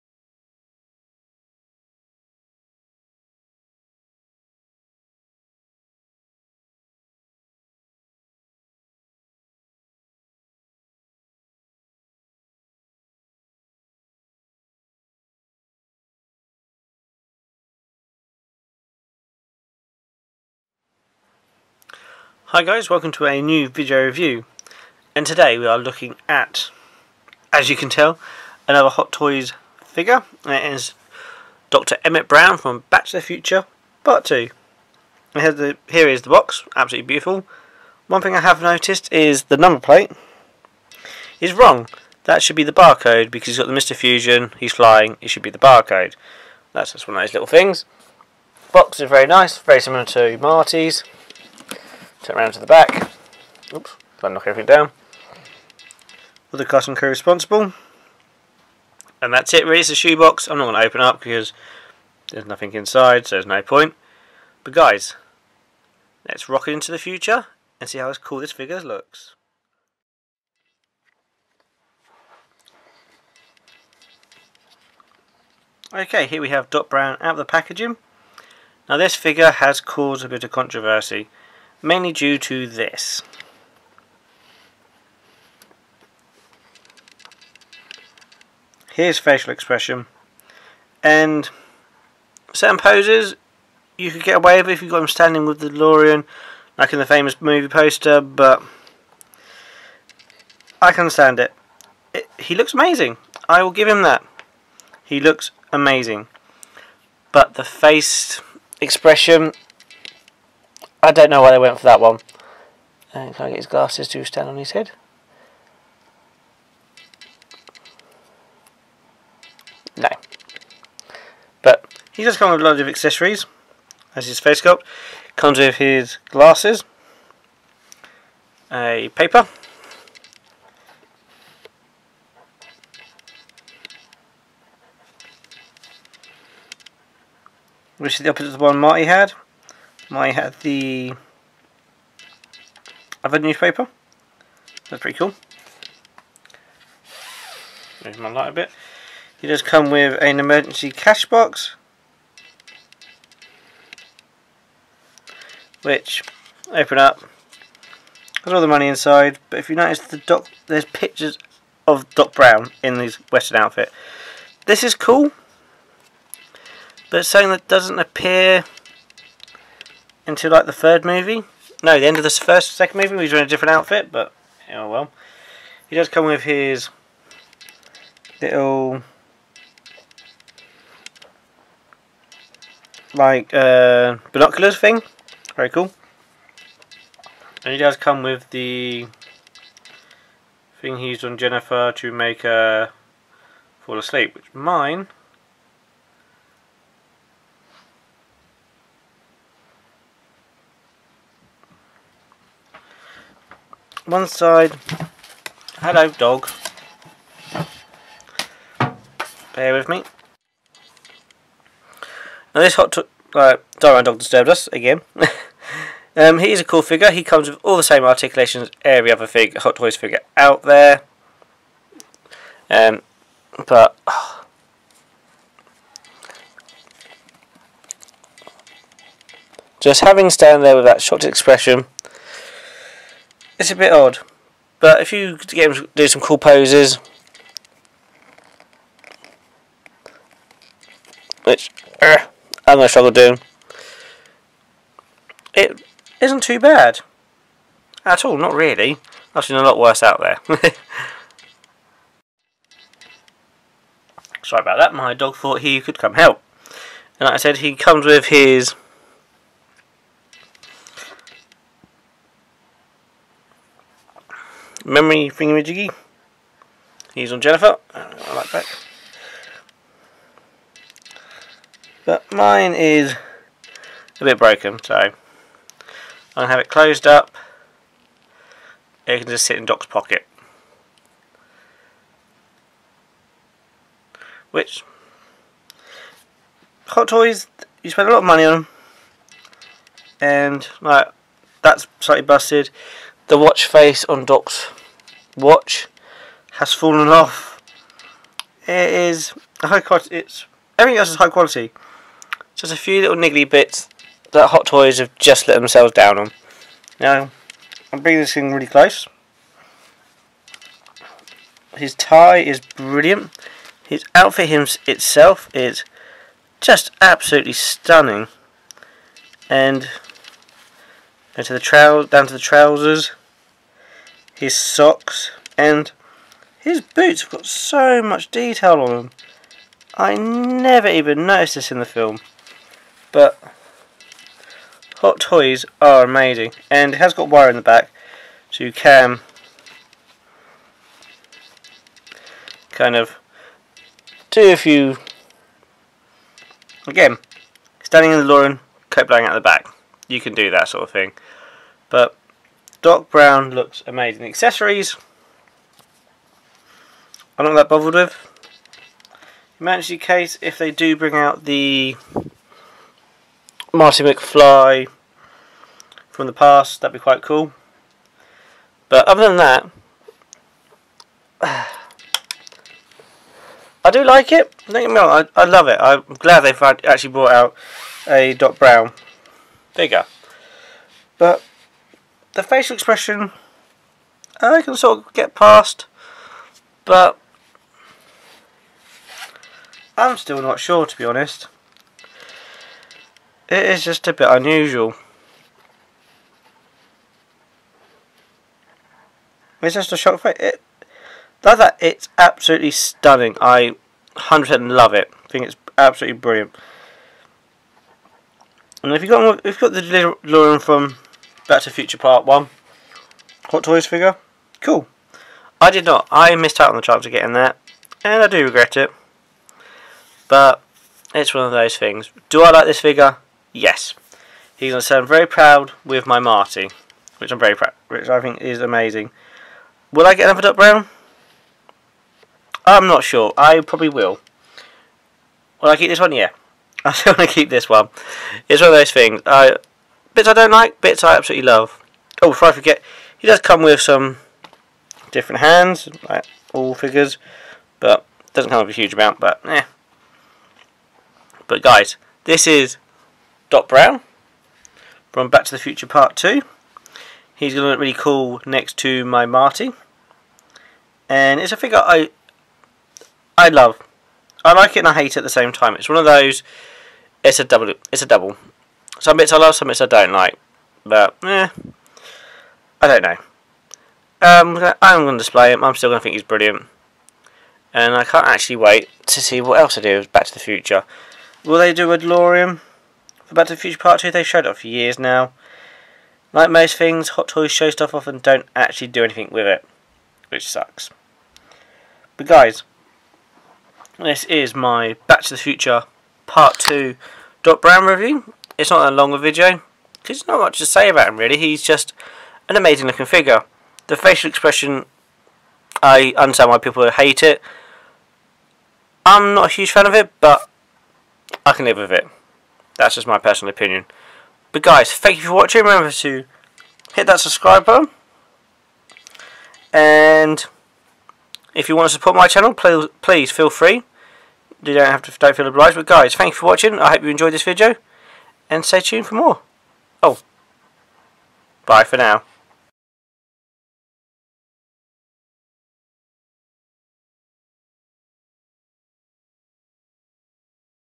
Hot Toys Back To The Future Part 2 Dr Brown Review
Hey everyone, here it is finally my Hot Toys Back to the Future Part 2 Doctor Emmit Brown. This is another 1:6 scale figure & wow what an amazing figure. Just amazing details & another figure im very happy to add to my collection.